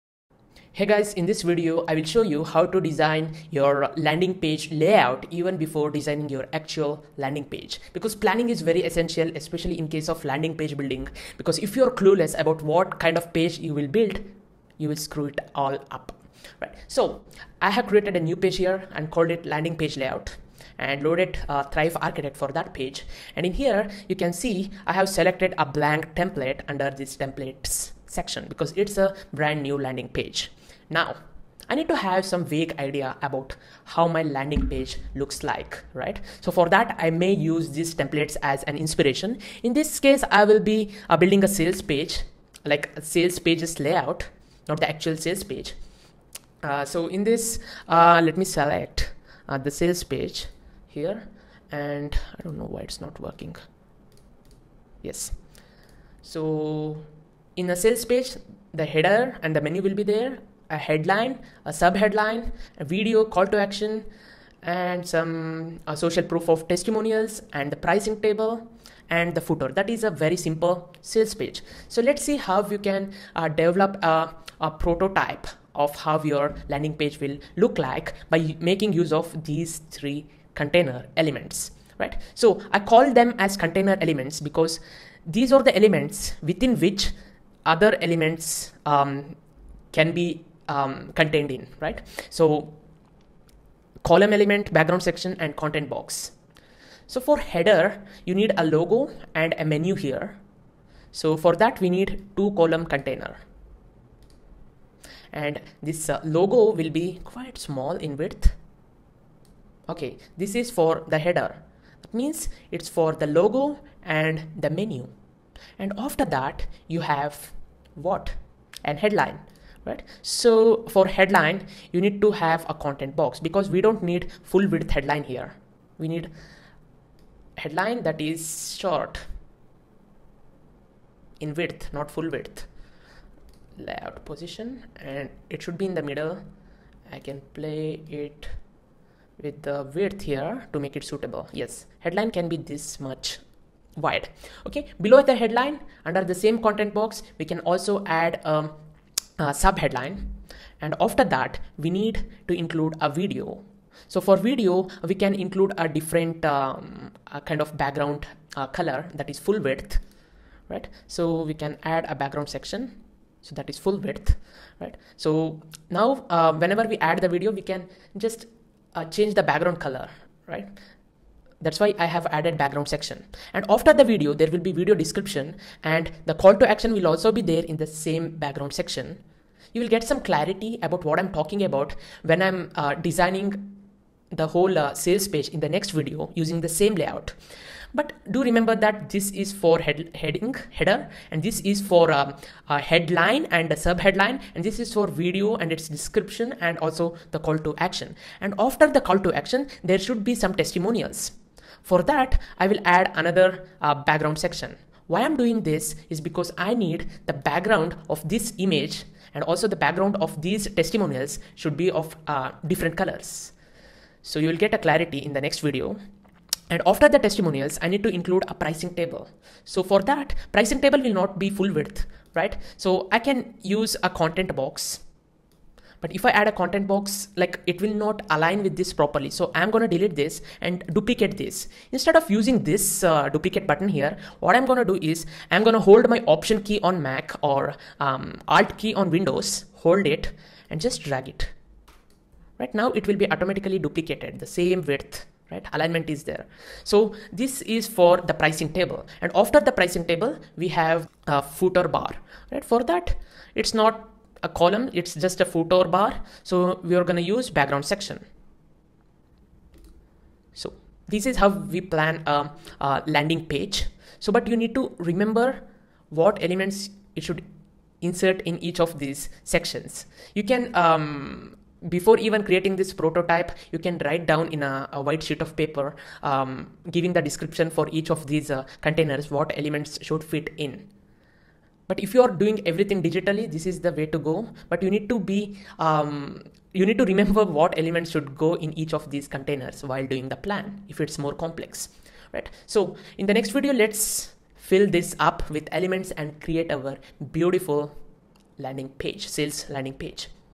Thrive Architect: #5: Creating a Structure of Your Landing Page
In this video, I'll teach you how to create a structure plan for your landing page.

Hope you guys found this video helpful.

► Do you need help growing your blog?